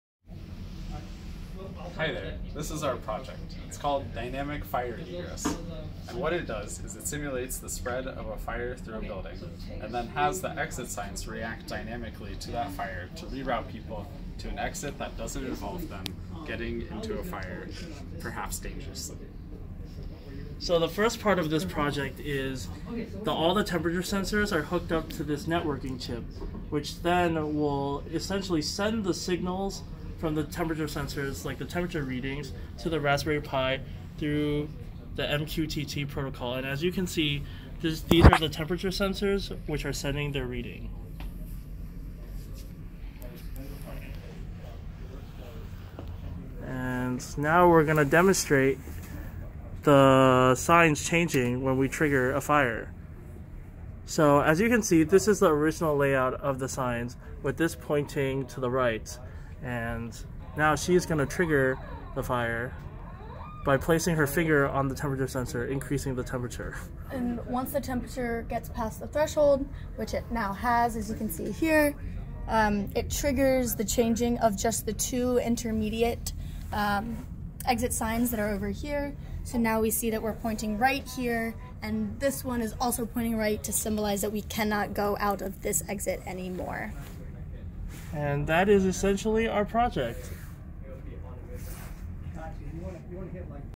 Dynamic Fire Egress
In case of an emergency, navigating to the nearest and safest exit can prove to be difficult when you are in an unfamiliar location or you don’t know the location of the nearest exit that avoids the emergency event. The Emergency Egress System provides topological guidance to individuals in a building to navigate to the nearest and safest emergency exit through the use of LED arrows attached to exit signs. Through an expanded emergency exit sign design, the addition of arrows placed to the left and right of the sign, individuals are able to receive real-time guidance for which exit(s) are on the shortest and safest path out, and which exits are not.